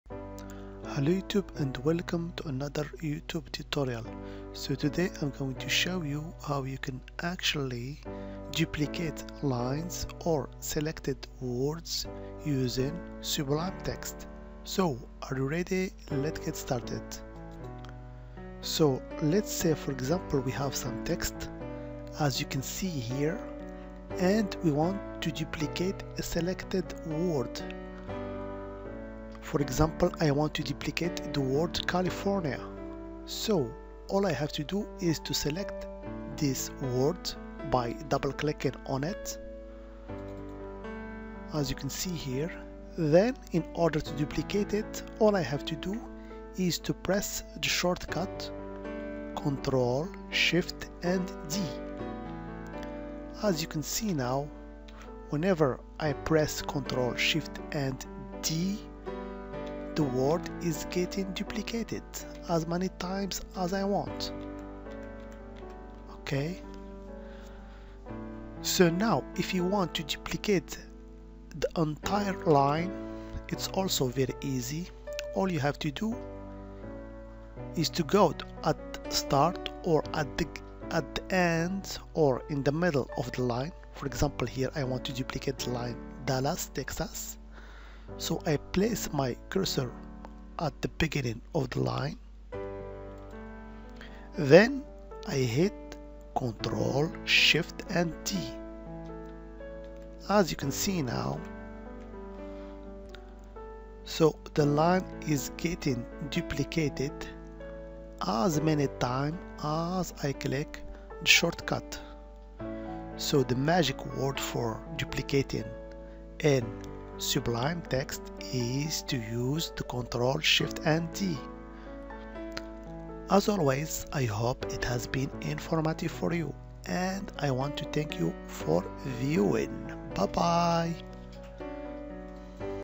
Sublime Text : How to Duplicate Line/Selected Text
In this simple video tutorial you will learn how to Duplicate a Line or Selected Text using a keyboard short-cut CTRL+SHIFT+D